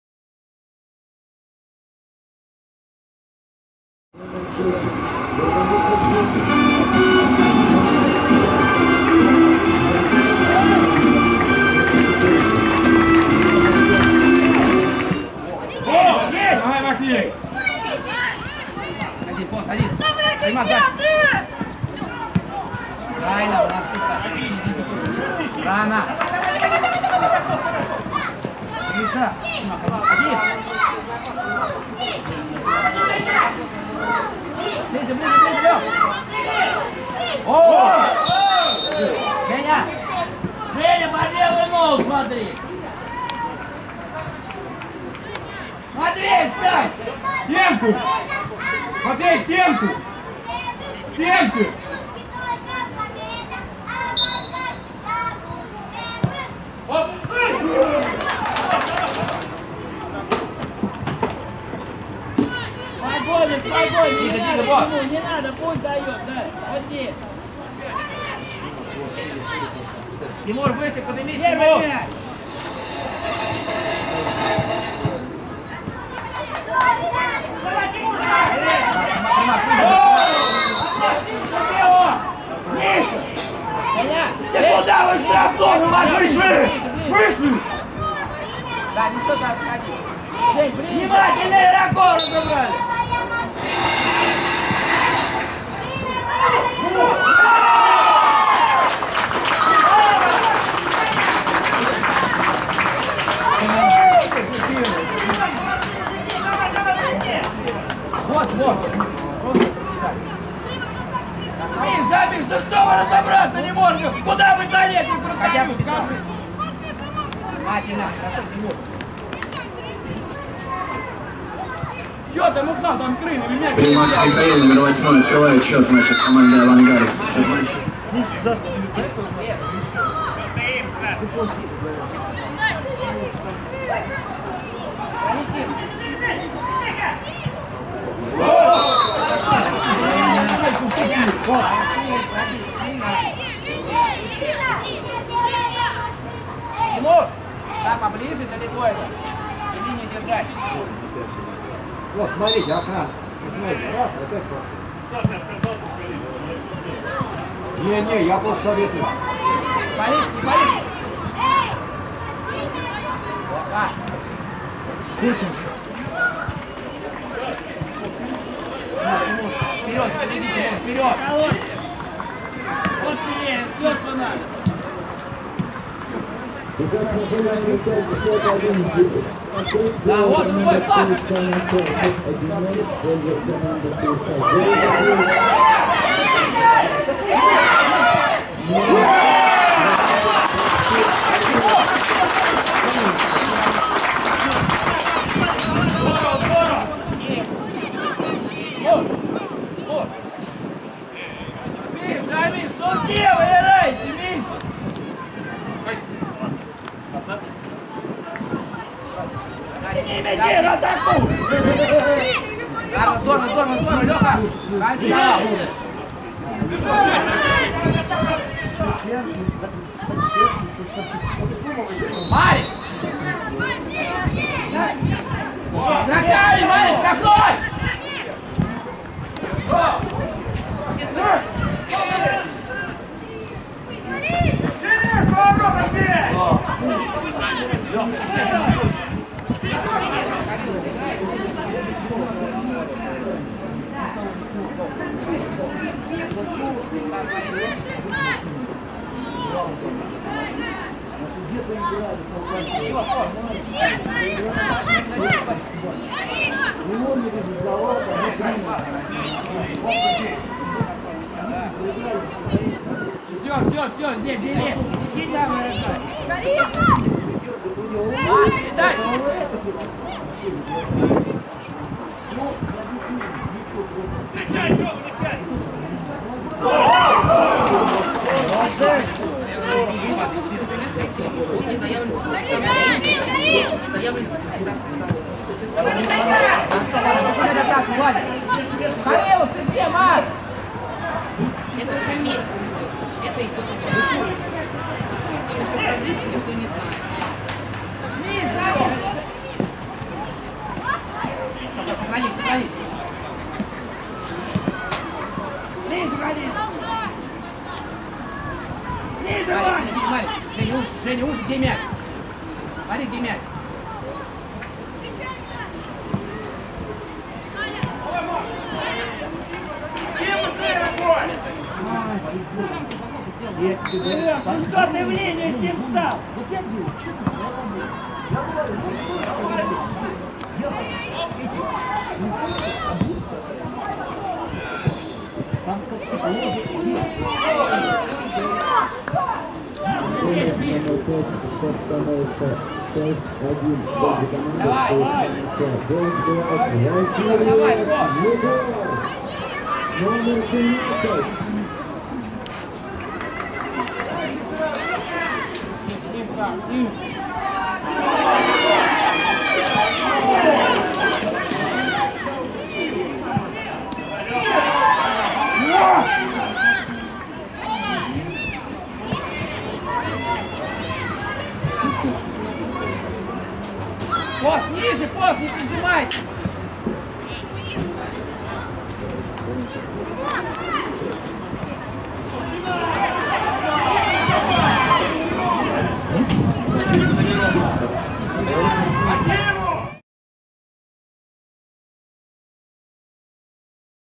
Обзор матча "Спартак" (2005 г. р.) - "Авангард"
Детский кубок чемпионов ЛУКОЙЛ
Команды 2005 года рождения
«Спартак» — «Авангард» — 2:0 (2:0)
Голы: Примак (6), Ким (13).
«Спартак»: Быков (Голубь, 43), Пальчиков (Кондратьев, 29), Носатый, Бозов (Красилин, 43), Постников (к), Климков, Ронжин (Степанов, 43), Филиппов (Гордеев, 43), Халилов (Давыдов, 38), Примак (Павлов, 36), Ким.